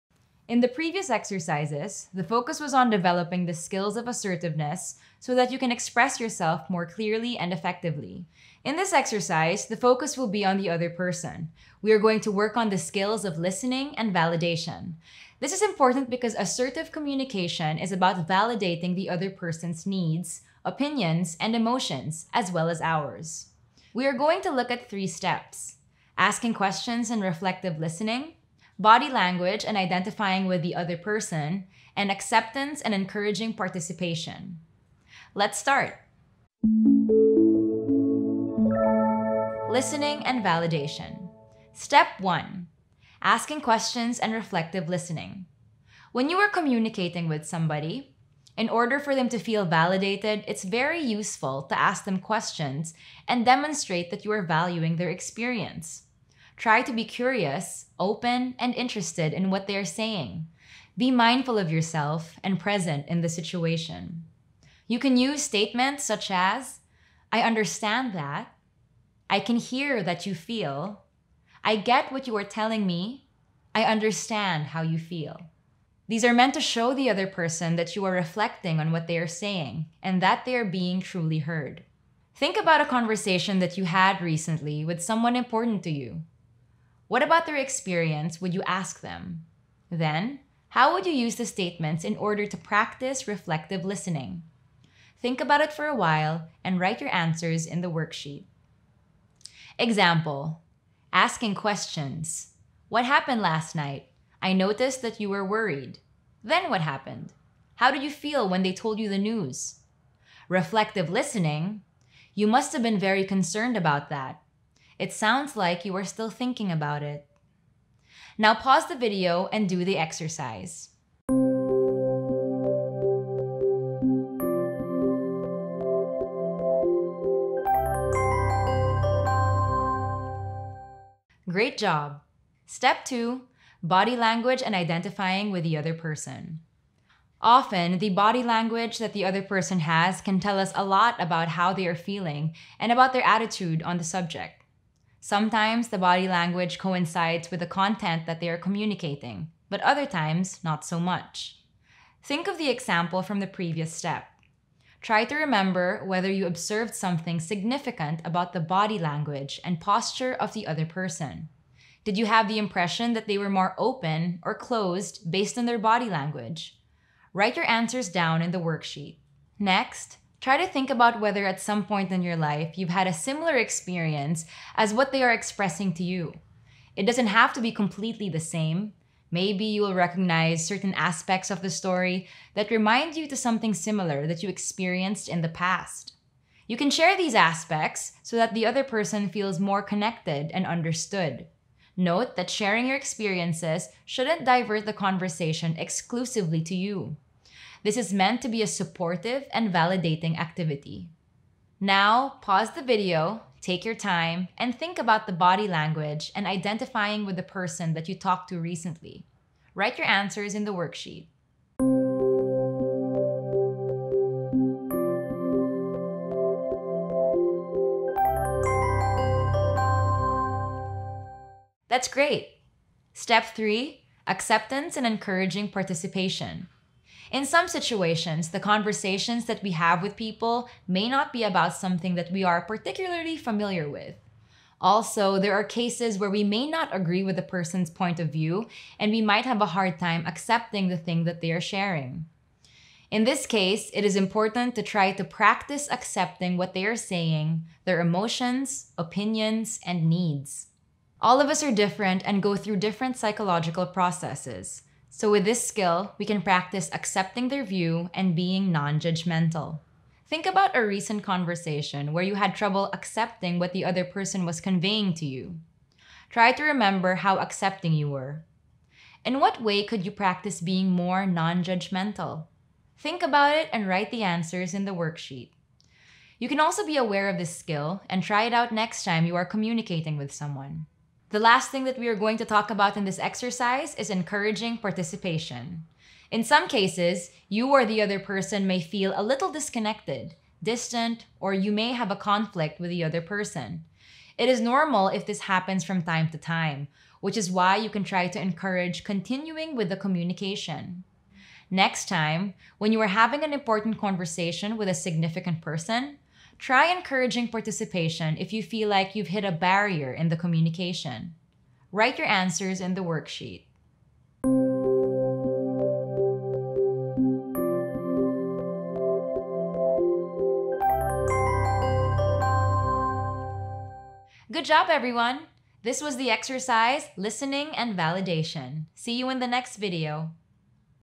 DBT Interpersonal Effectiveness #5: Listening and Validation
In this exercise we will focus on several aspects of attentive and mindful listening and ways of validating what the other person is telling us. Interpersonal effectiveness requires that we acknowledge the other person as well as ourselves. Sometimes, when we are busy doing everyday activities, it is not difficult to overlook the other person's day, their concerns, opinions and feelings.